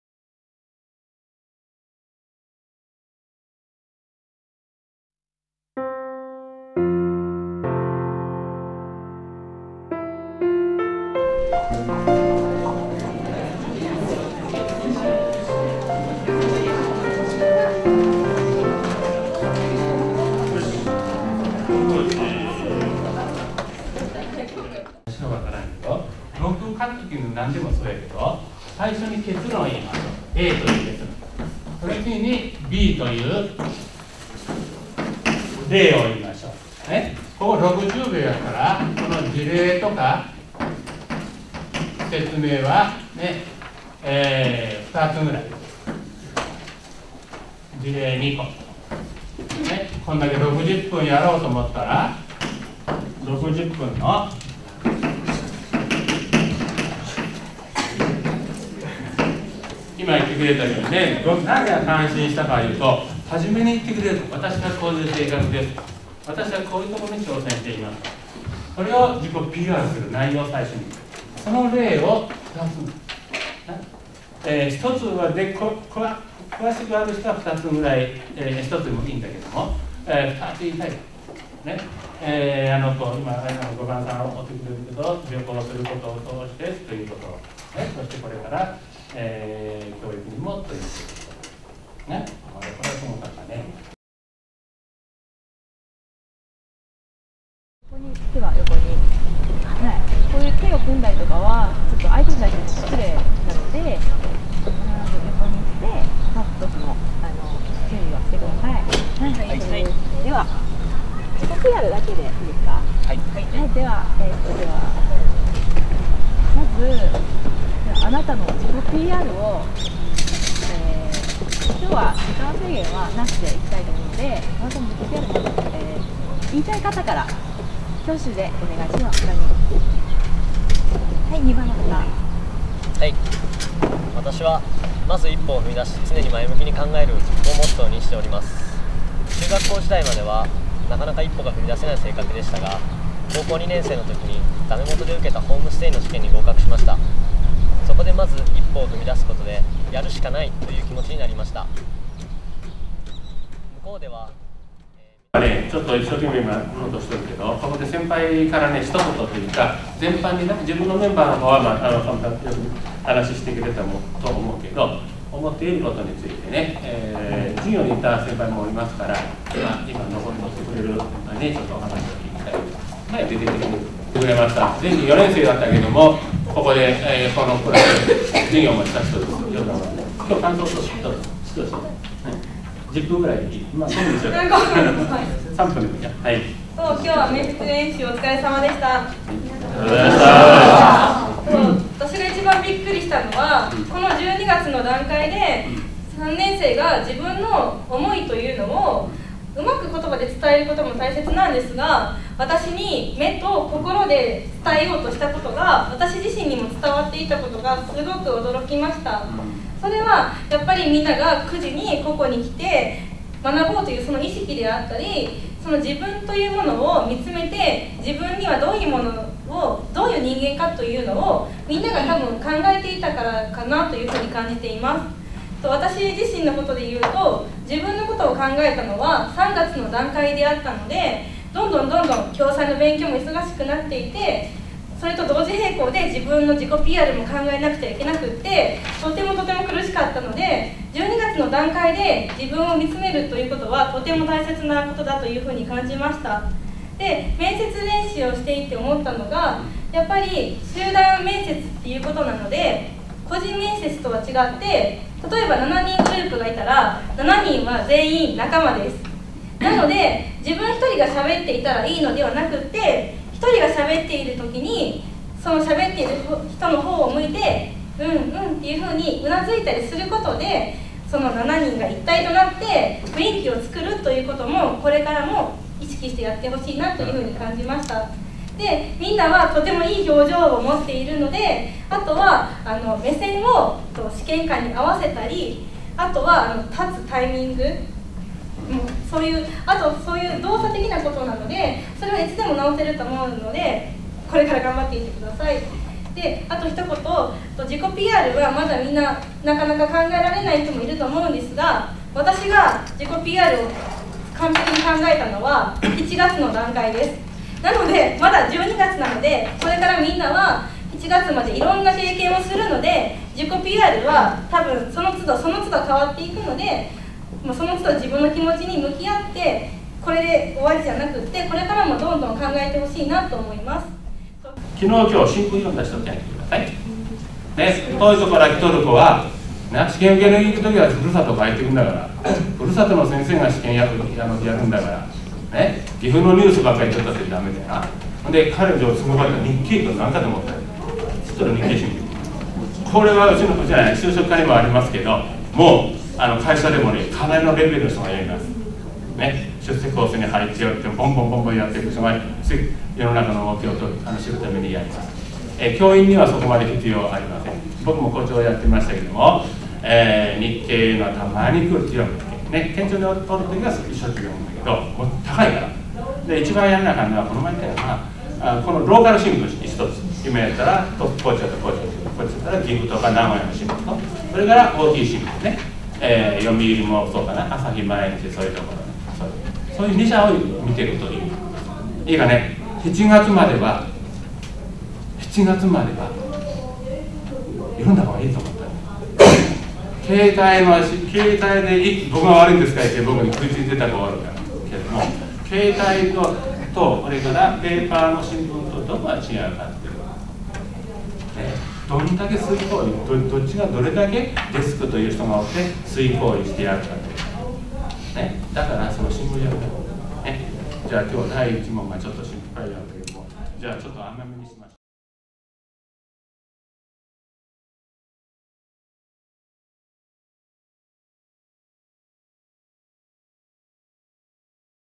教員採用試験合格を目指す～教師への道～
岐阜聖徳学園大学で"教師への道"が行われました。去年の10月から3年生を対象とした教員採用試験の概要と求められている教師像について古澤哲男講師による授業が行われています。
※ 現在は4年次に開講しています。